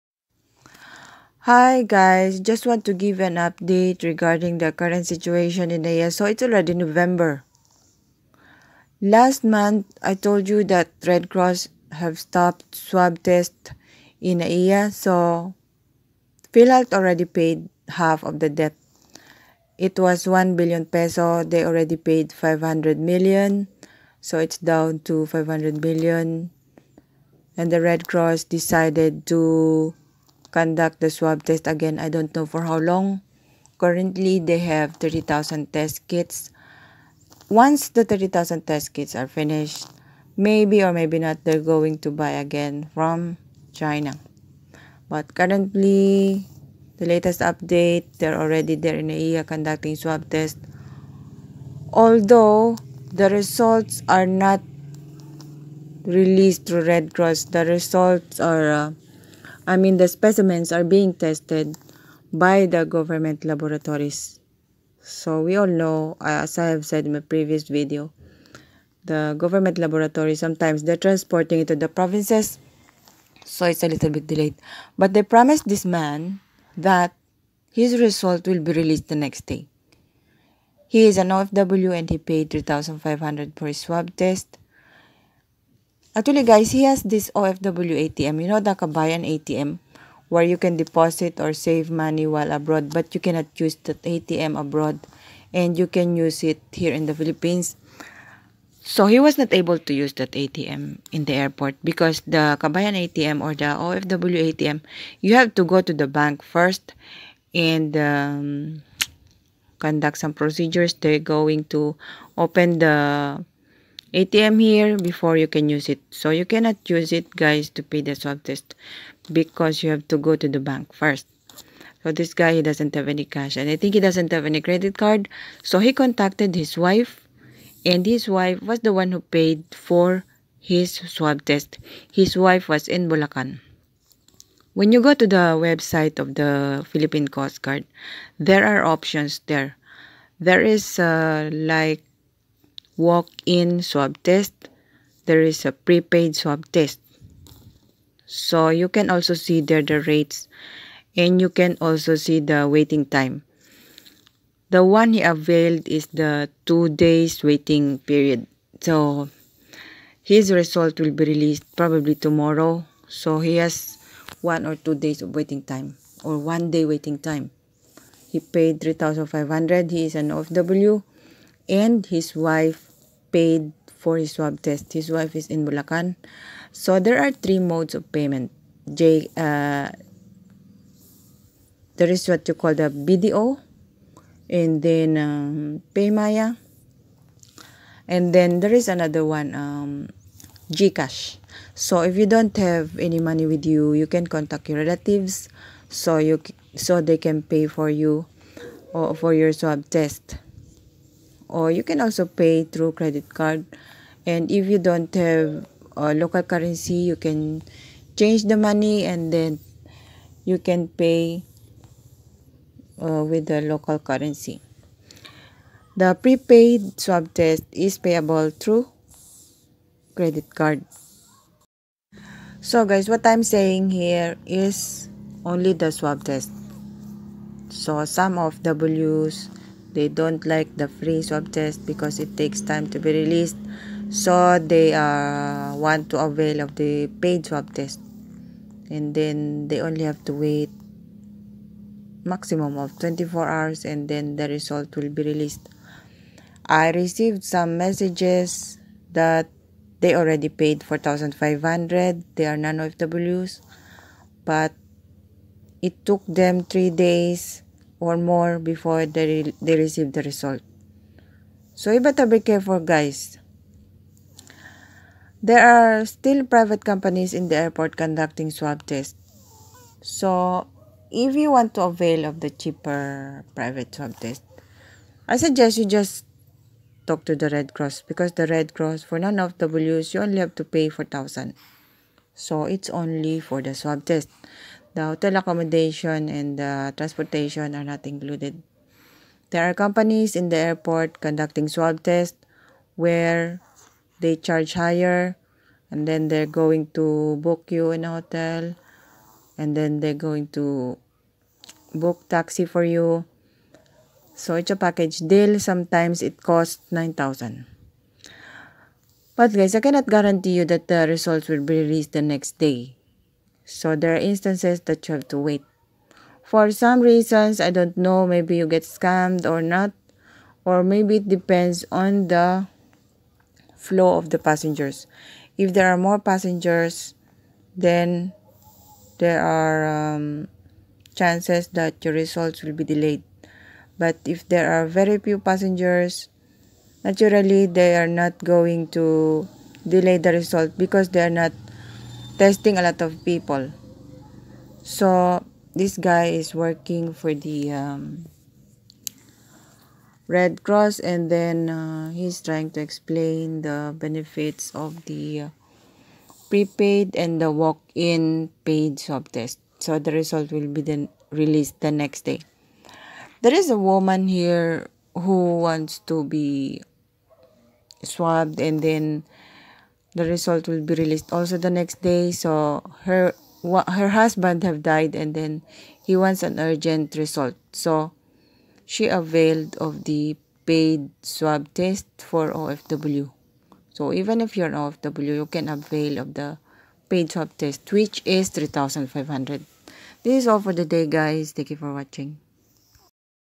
Swab Tests In Philippine Airports
Correcrion: QR code po ang ipiprint at pipicturan galing sa website. Ang barcode ay sa airport na ibibigay. Anim na sticker sya
pakipanood po ang video sa quaarantine facility ng 2Go. Iclick lamang ang link na ito:

Mag register sa website bago umuwi ng Pilipinas para mapabilis ang proseso sa airport.
Wag kalimutan iscan ang passport o picturan dahil kailangan mo itong iupload